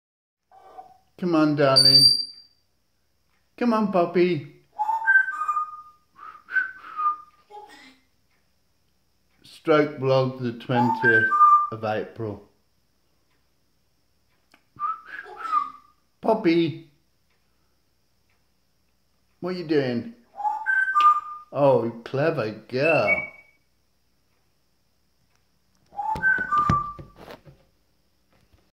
Stroke blog 20th April
Last night  I carried a coffee  through for my wife  I went on line with stroke focus survivors in America this morning I got up as usual got our breakfasts ready ate mine took the  bottles through brought 2 bottles of non alcoholic beer  back for my wife will check the wood open my greenhouse  and get my own lunch